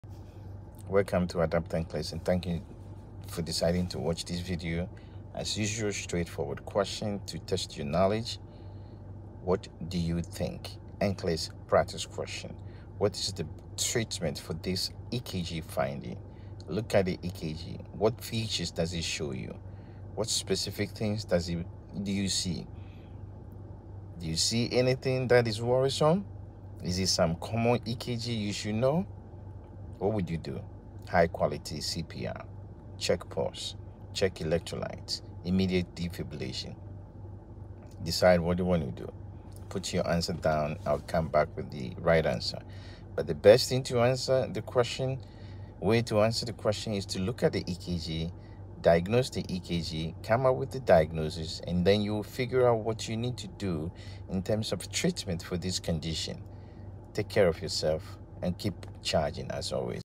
NCLEX Practice Question: EKG NCLEX Question and Answer | ADAPT NCLEX
NCLEX Practice Question | NCLEX Question and Answer RN LPN | ADAPT NCLEX.
Quick EKG review for nclex using high yield nclex practice question with rationale.

Let's test your knowledge! What is the treatment for this EKG finding? Take a look at the EKG, analyze the features, and tell us what you see regarding the EKG.

This video presents a straightforward NCLEX practice question designed to test your knowledge of EKG interpretation. We encourage you to analyze the specific features of the ecg and identify the correct treatment for the presented finding. This is essential medical education for anyone preparing to understand various heart rhythms and how to read ekg effectively.